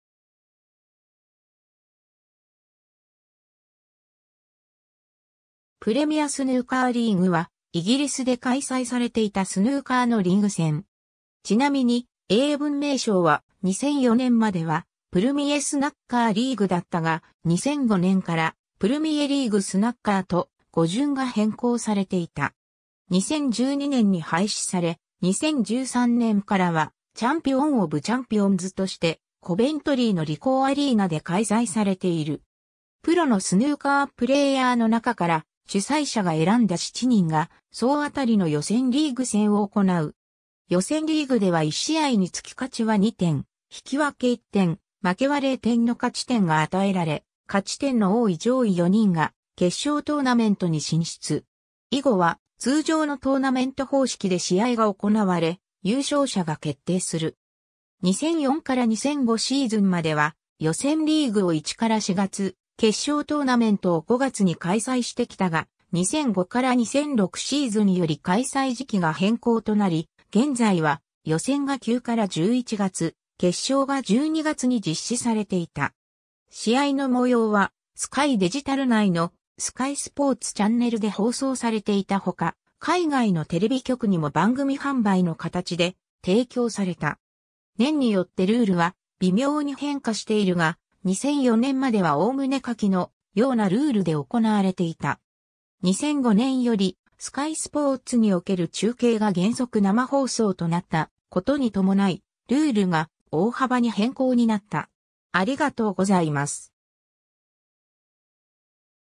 プレミアスヌーカーリーグ
プレミアスヌーカーリーグは、イギリスで開催されていたスヌーカーのリーグ戦。
ちなみに英文名称は、2004年までは「Premier Snooker League」だったが、2005年から「Premier League Snooker」と語順が変更されていた。
2012年に廃止され、2013年からはチャンピオン・オブ・チャンピオンズとしてコヴェントリーのリコー・アリーナで開催されている。
 プロのスヌーカープレーヤーの中から、主催者が選んだ7人が総当りの予選リーグ戦を行う。
予選リーグでは1試合につき勝ちは2点、引き分け1点、負けは0点の勝ち点が与えられ、勝ち点の多い上位4人（勝ち点が同点で並んだ場合は勝利フレーム数の多い順）が決勝トーナメントに進出。
以後は通常のトーナメント方式で試合が行われ優勝者が決定する。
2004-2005シーズンまでは予選リーグを1～4月、決勝トーナメントを5月に開催してきたが、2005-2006シーズンより開催時期が変更となり、現在は予選が9～11月、決勝が12月に実施されていた。
 試合の模様はSky Digital（旧BスカイB）内の「Sky Sports」チャンネルで放送されていたほか、海外のテレビ局にも番組販売の形で提供された。
 年によってルールは微妙に変化しているが、2004年までは概ね下記のようなルールで行われていた。
 2005年よりSky Sportsにおける中継が原則生放送となったことに伴い、ルールが大幅に変更になった。